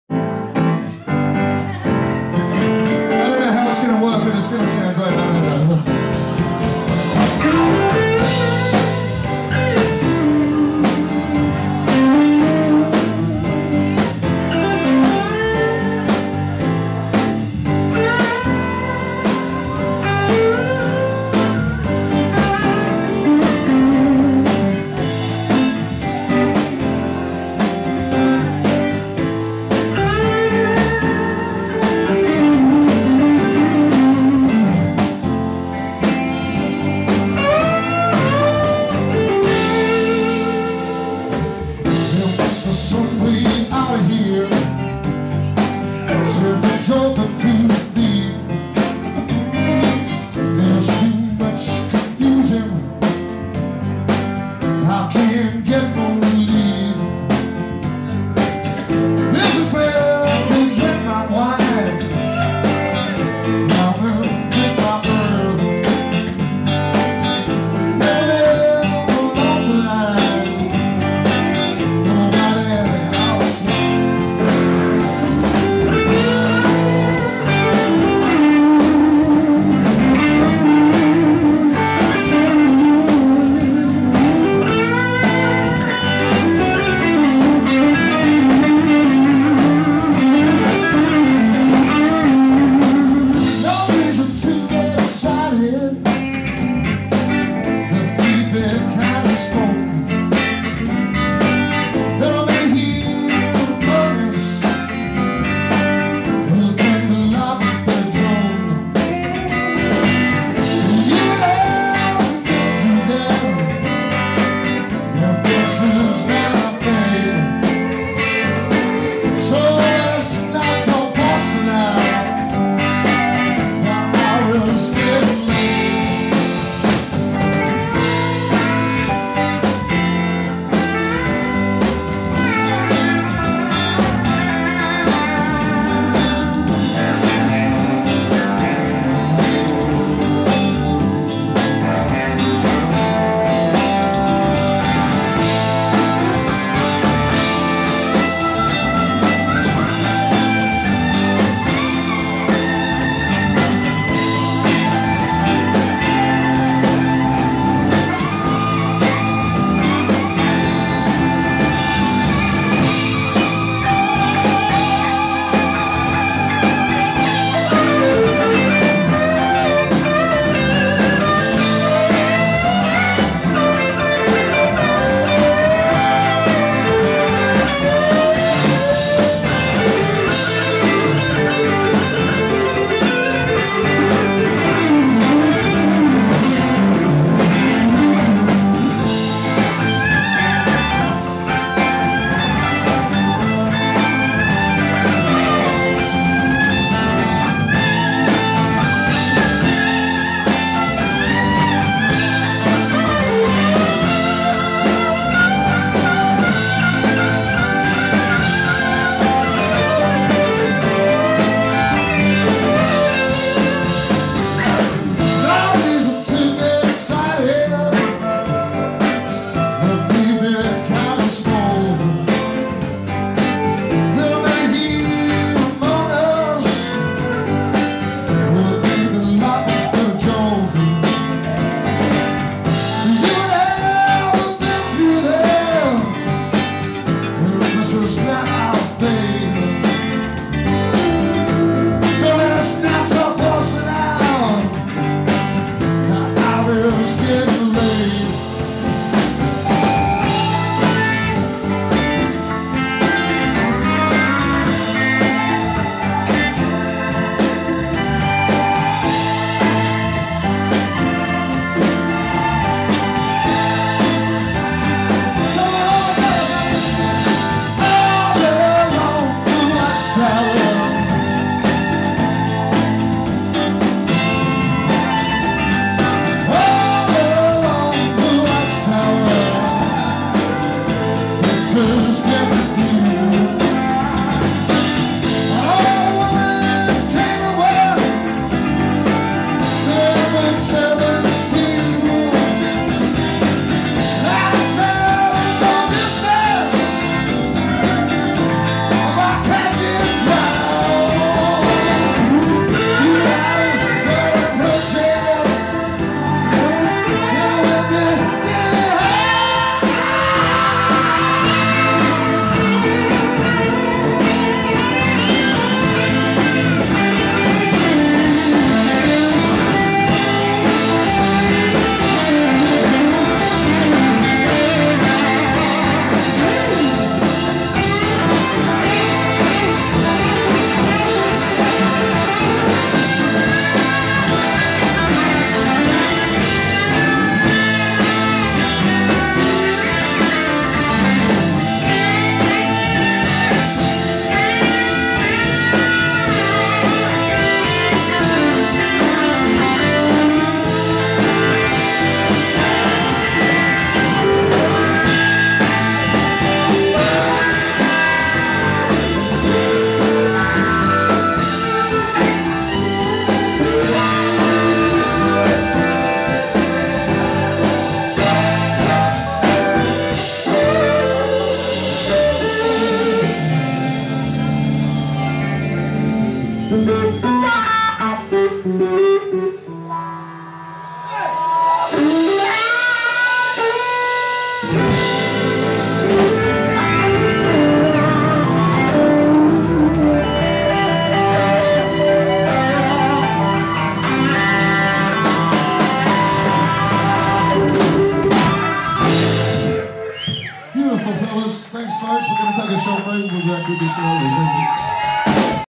MITCH ANDERSON BAND
MITCH ANDERSON BAND LIVE @ sando hotel newtown sydney australia.
MITCH ANDERSON - keys/vox. BEN ISACKSON - drums. JAMES HASELWOOD - bass. ADAM PRINGLE - guitar. july 2010